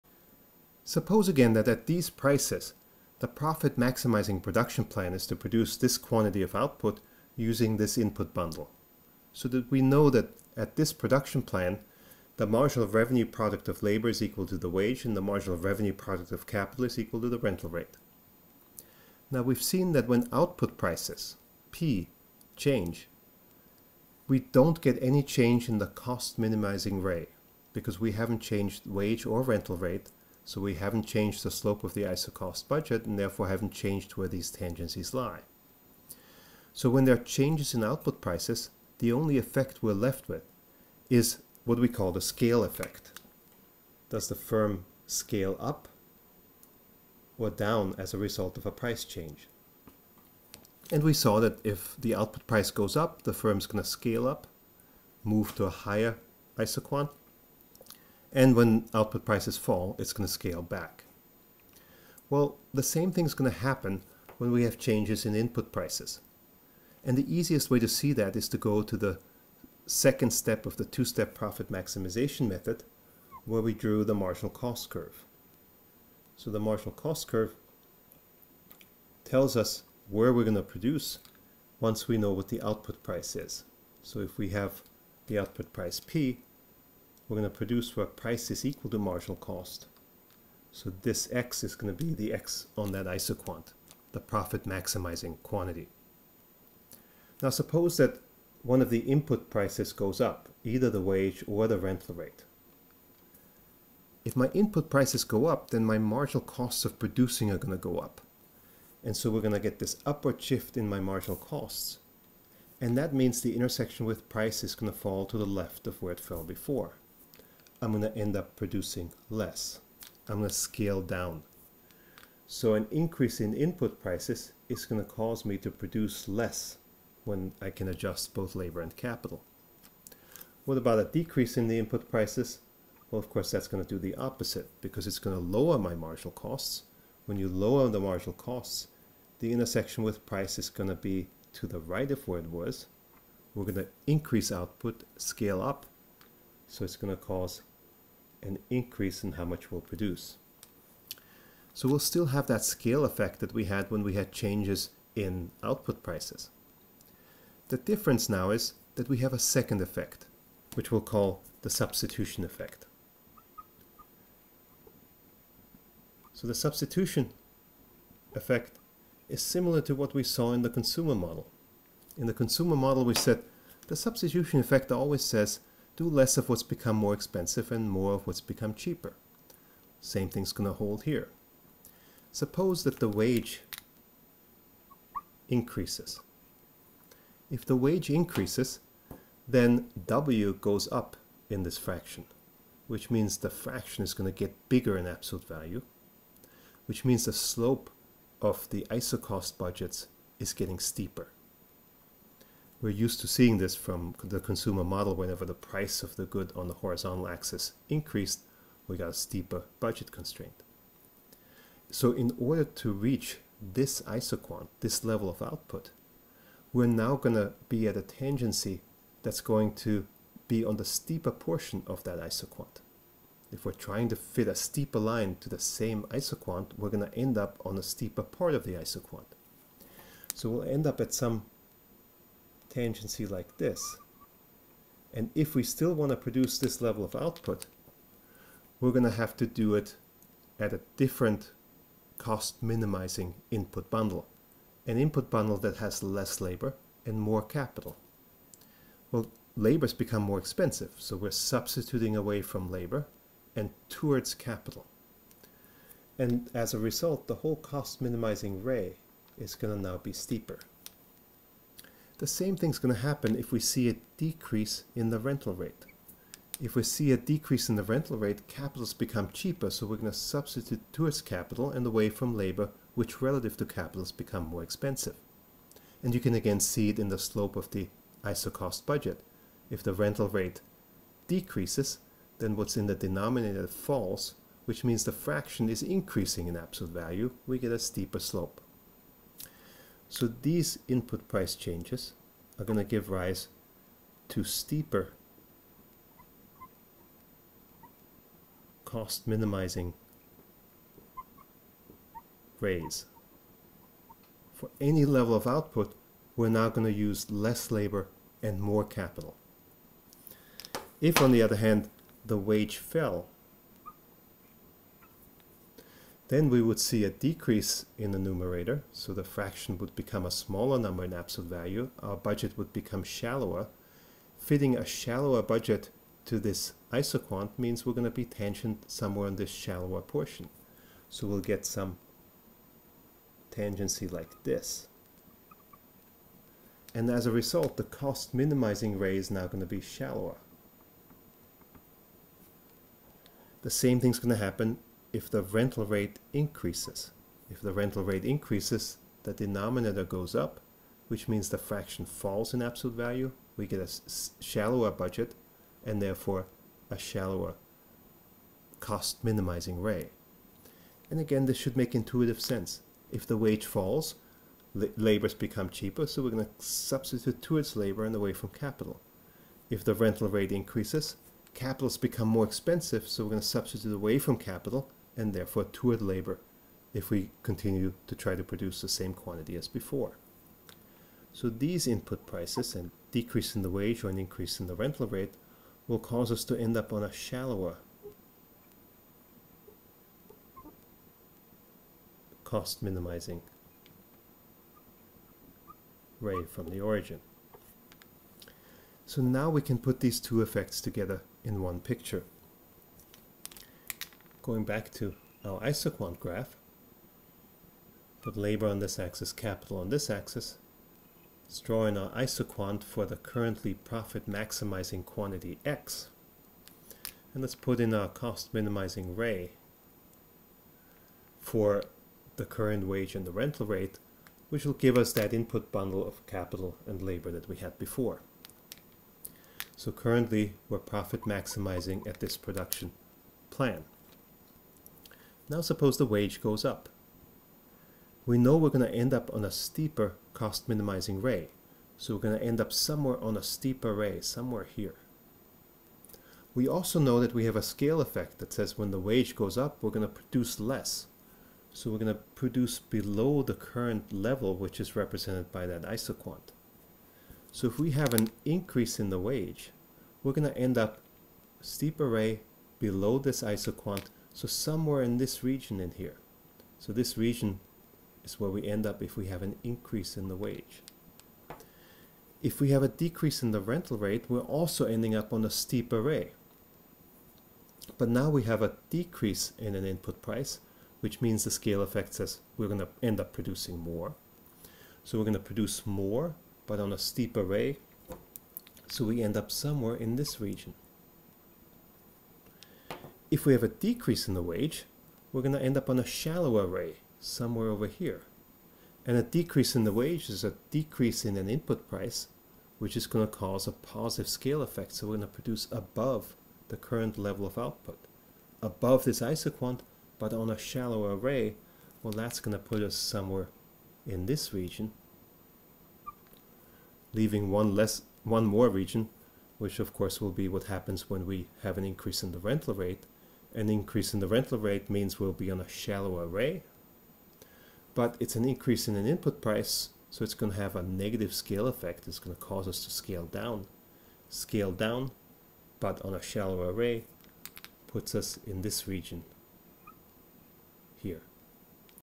13.4.Input Prices and Substitution Effects
This video illustrates substitution effects in production -- effects that emerge when input prices change and firms switch to different cost minimizing rays (implying new ratios of labor to capital).